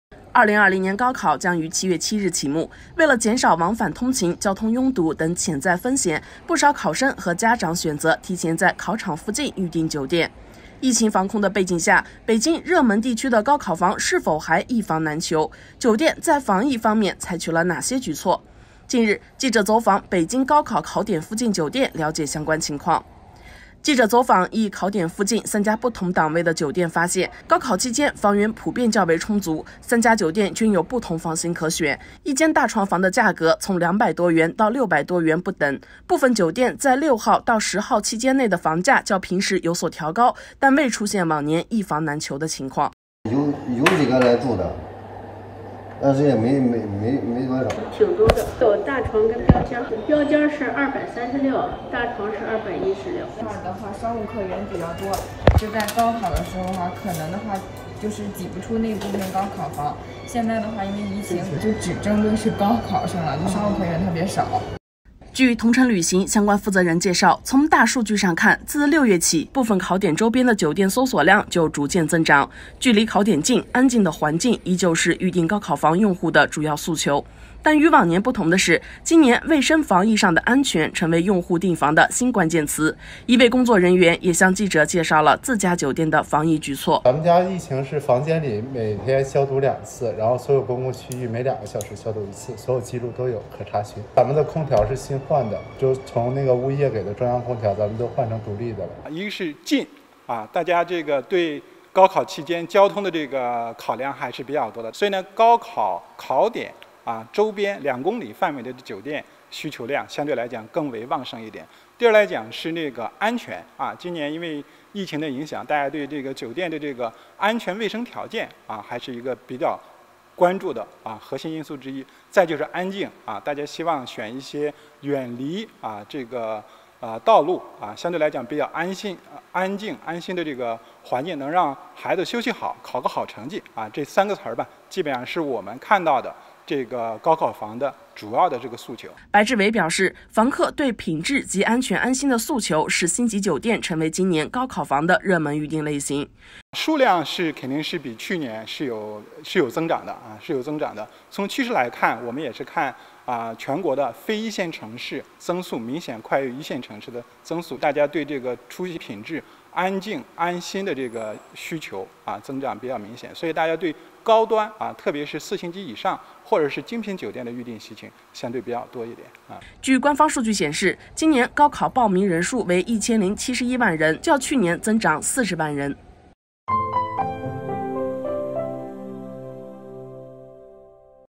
特殊之年 “高考房”还是“一房难求”吗？
2020年高考在即，为减少往返通勤、交通拥堵等潜在风险，不少考生和家长提前在考场附近预订酒店。疫情防控背景下，北京热门地区高考房是否还“一房难求”？酒店采取了哪些防疫举措？近日记者走访北京高考考点附近酒店了解相关情况。